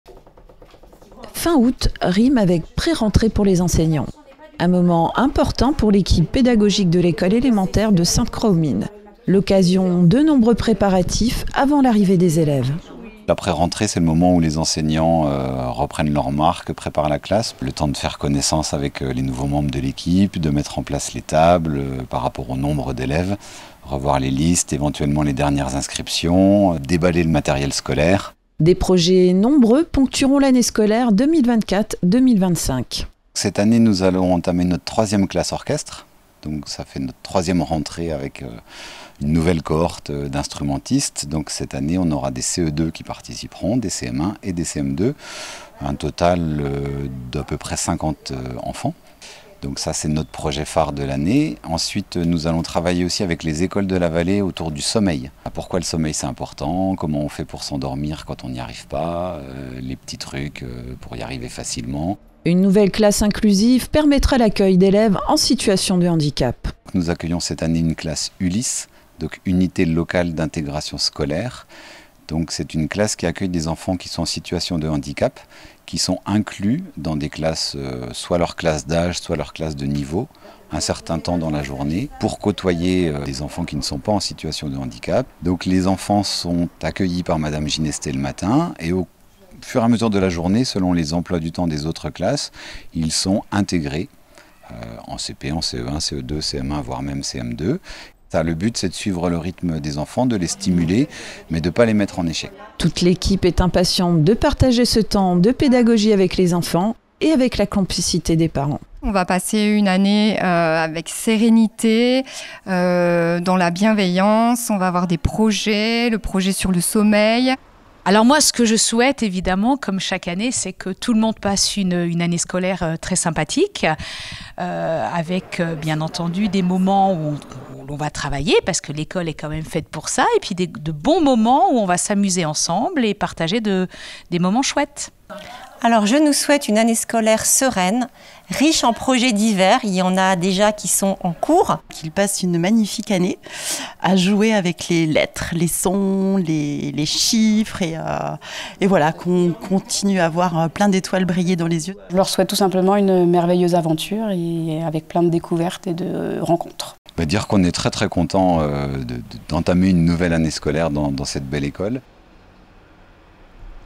Pré-rentrée de l'école primaire de Sainte-Croix-aux-Mines
L'école primaire de Sainte-Croix-aux-Mines a effectué sa pré-rentrée vendredi 30 août.  Un temps d'échange entre professeurs a permis d'évoquer les projets de la nouvelle année scolaire.